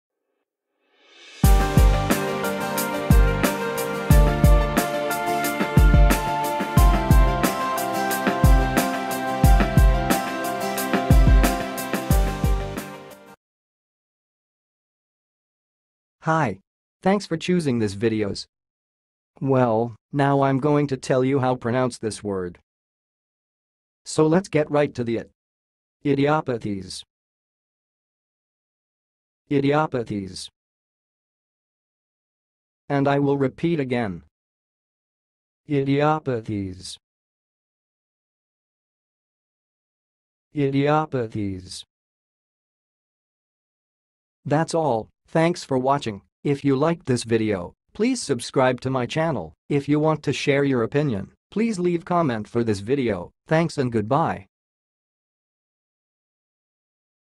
IDIOPATHIES - HOW TO PRONOUNCE IT!?
Watch how to say and pronounce "idiopathies"!
Want to know how other words sound like? Look for idiopathies, idiopathy on wordbox channel!